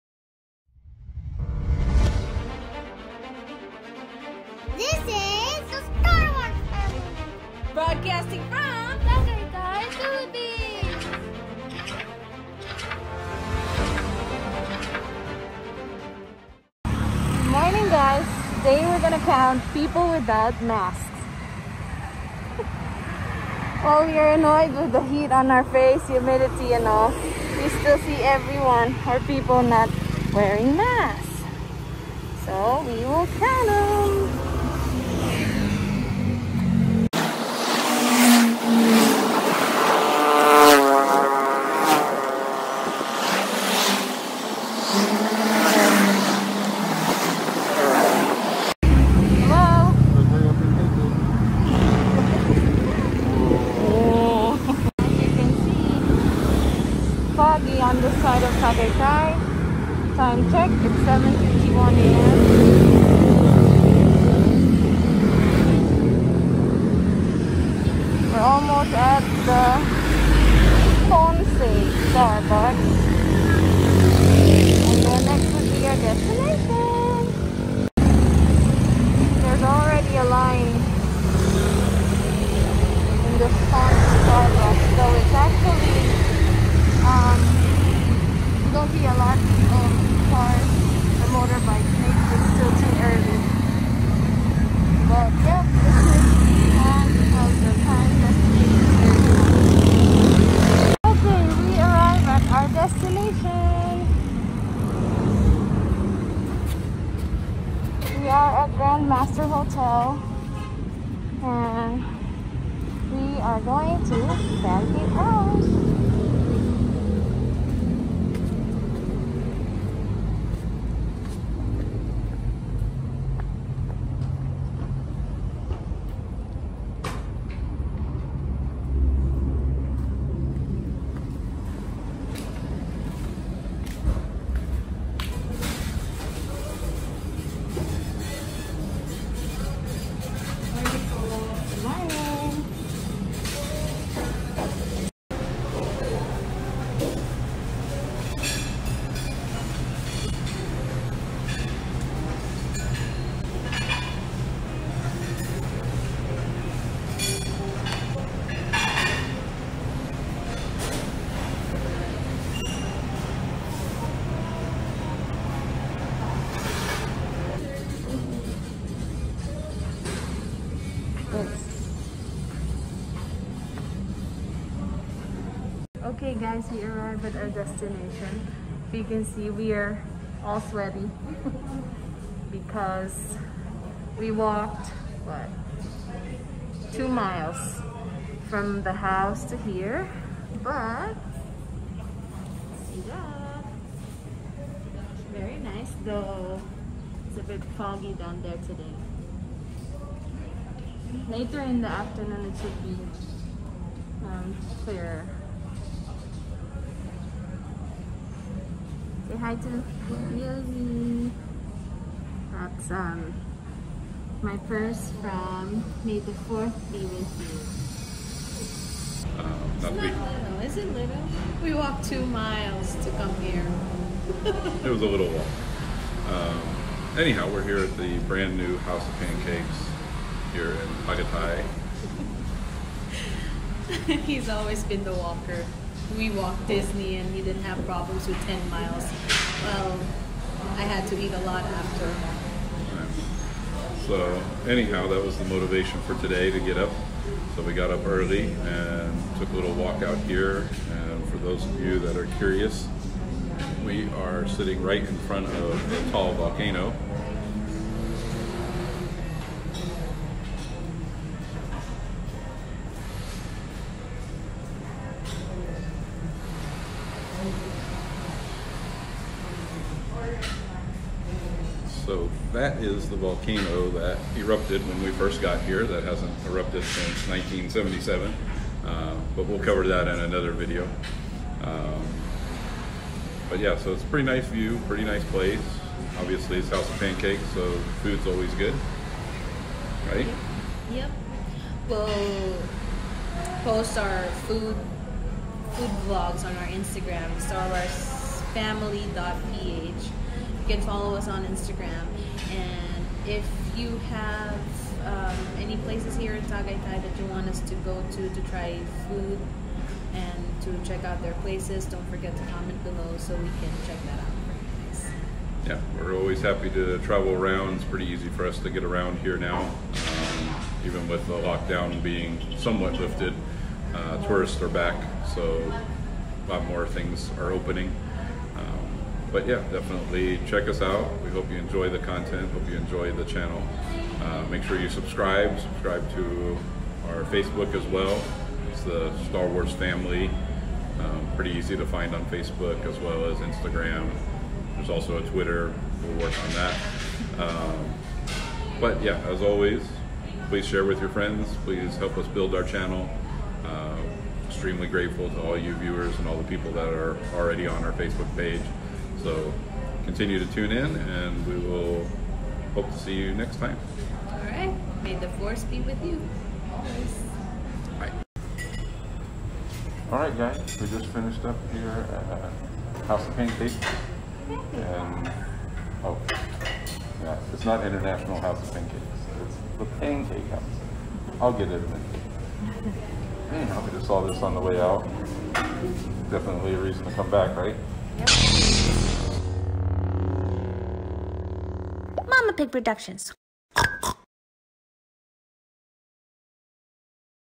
Star Wars Family Eats: NEW Pancake House in Tagaytay. What a view of Taal Volcano and Taal Lake!
The Grandmaster Hotel in Tagaytay now has a Pancake House inside. We take a walk to check it out.
SPOILER: One of the best views in Tagaytay

We’re also posting updates on our Facebook Page, like it here!